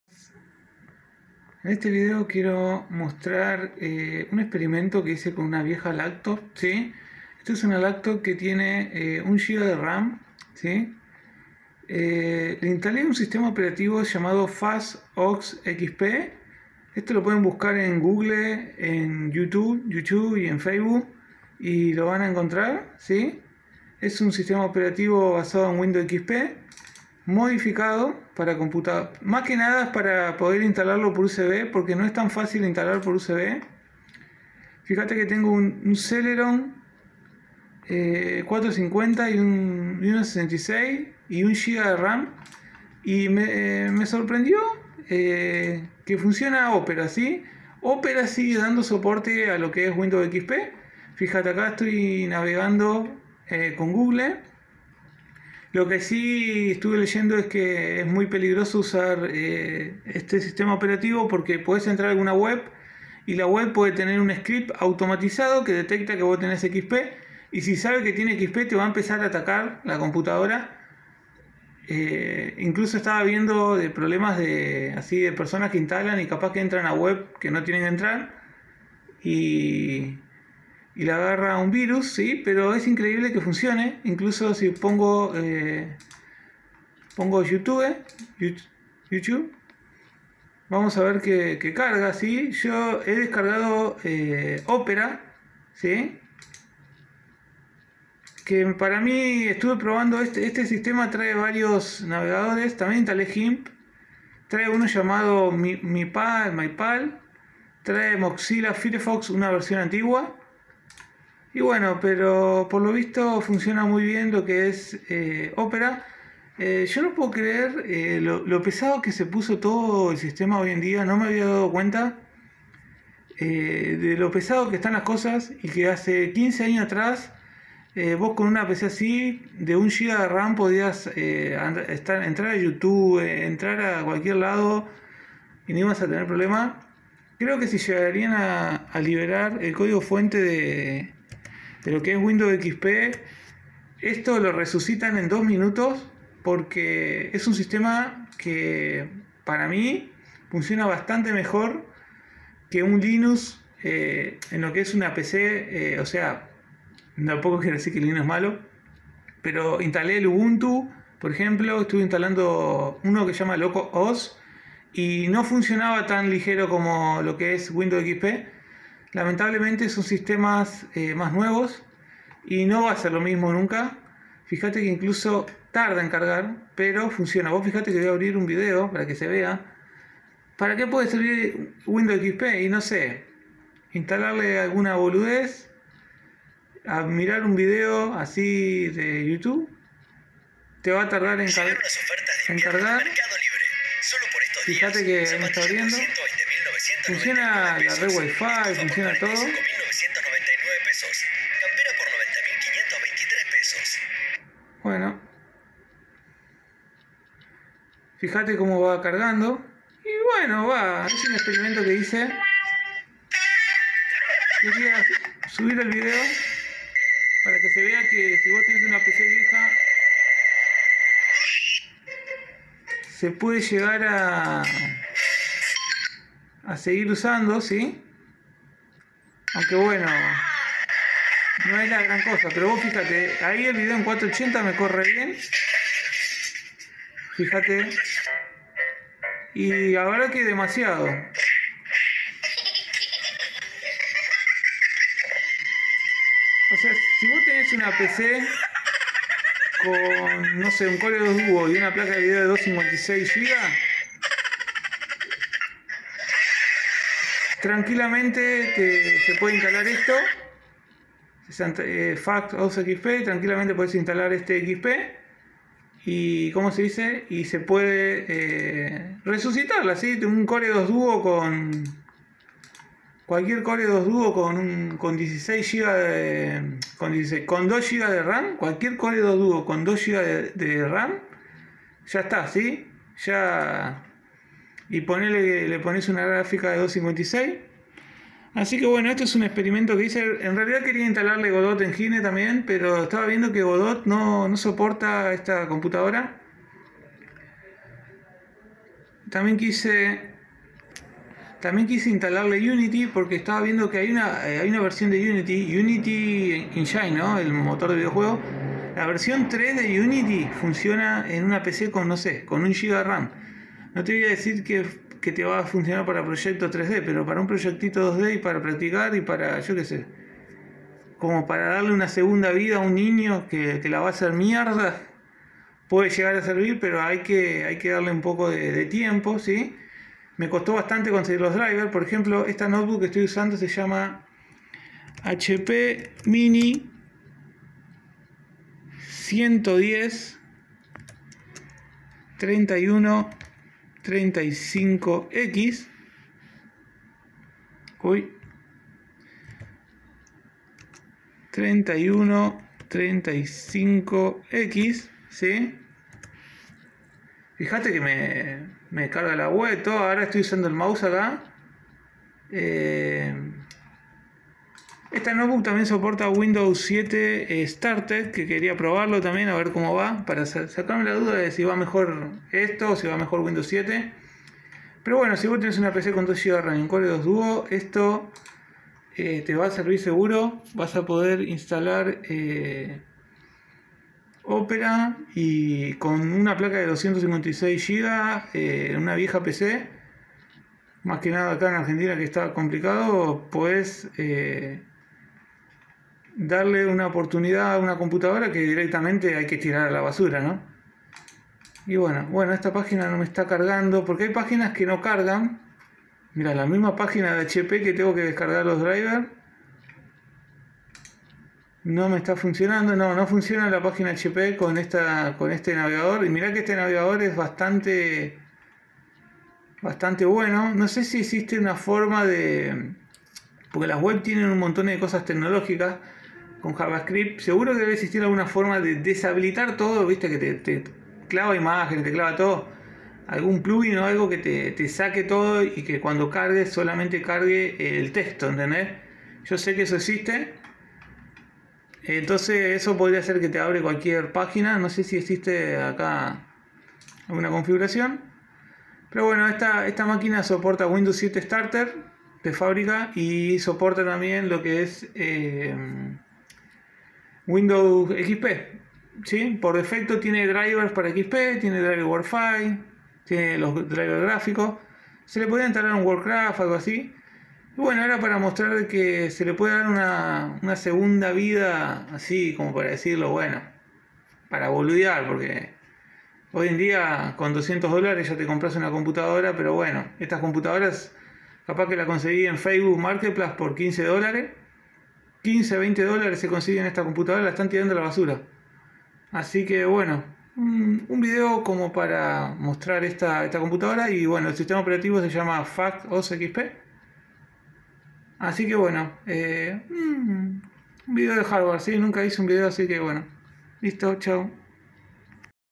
Windows XP en 2024 en HP Mini 110-3135dx experimento con intel ATOM 1gb de ram y FAST OS XP
una pc con un intel atom y 1 gb de ram.